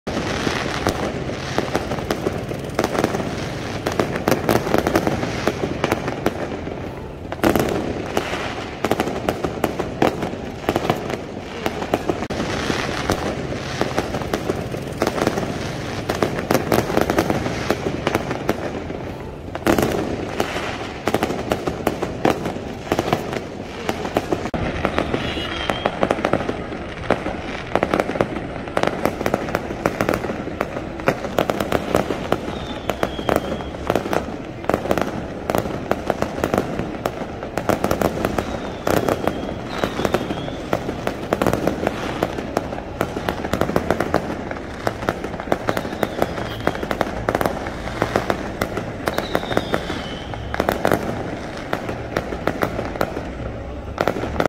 المولد النبوي الشريف
"سماء مدينة إب تضيء بالألعاب النارية احتفالاً بالمولد النبوي الشريف"

تصوير : محمد الجلال